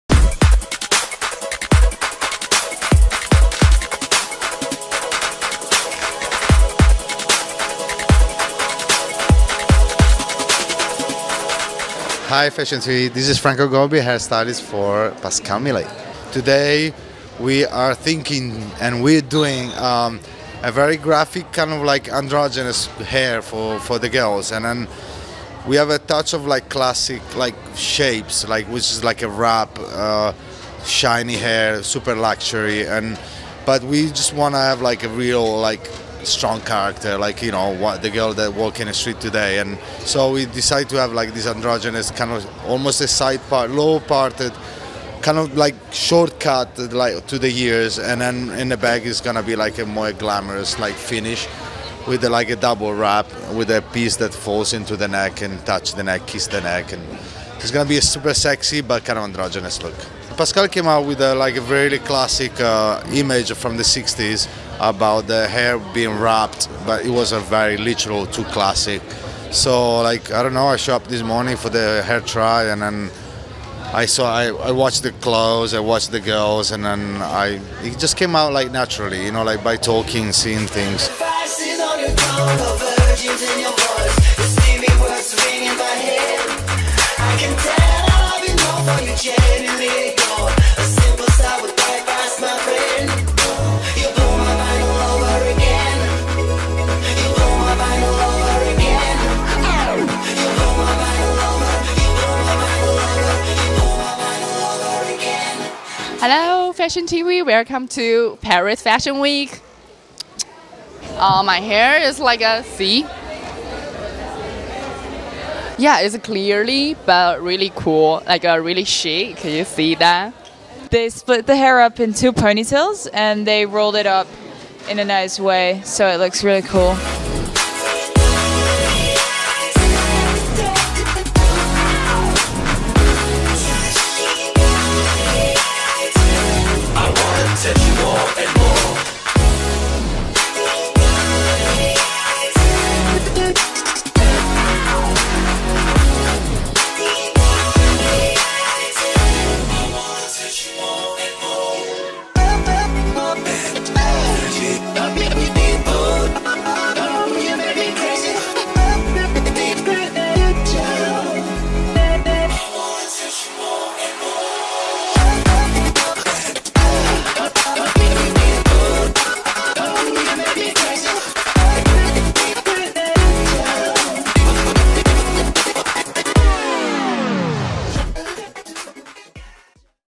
Hairstyle at Pascal Millet Fall/Winter 2016-17 Paris Fashion Week | FashionTV
Download the FashionTV+ app, and stay tuned:
PARIS - FashionTV goes backstage at Paris Fashion Week to bring you all the inside action direct from the artists and stylists as they prepare the models for the runway shows! Join FTV at Pascal MIllet Fall/Winter 2016-17 as we bring you an exclusive interview with hairstylist Franco Gobbi as he talks to us through the "androgenous" style hair look for the collection! with appearances by: Franco Gobbi, Gia Yi, Anniek Abma